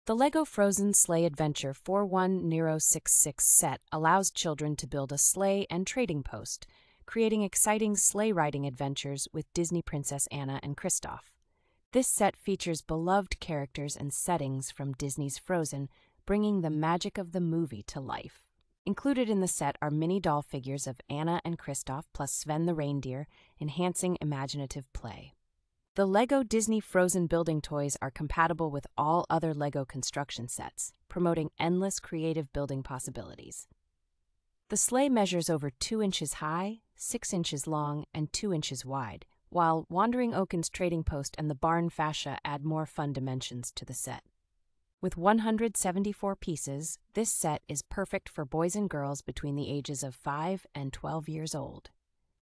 REVIEW (2025): Lego Frozen Sleigh Adventure 41066. ESSENTIAL details.
The Lego Frozen Sleigh Adventure 41066 set allows children to build a sleigh and trading post, creating exciting sleigh riding adventures with Disney Princess Anna and Kristoff.

This set features beloved characters and settings from Disney's Frozen, bringing the magic of the movie to life.

Included in the set are mini-doll figures of Anna and Kristoff, plus Sven the reindeer, enhancing imaginative play.

The Lego Disney Frozen building toys are compatible with all other Lego construction sets, promoting endless creative building possibilities.

The sleigh measures over two inches high, six inches long, and two inches wide, while Wandering Oaken's Trading Post and the barn fascia add more fun dimensions to the set.

With one hundred seventy-four pieces, this set is perfect for boys and girls between the ages of five and twelve years old.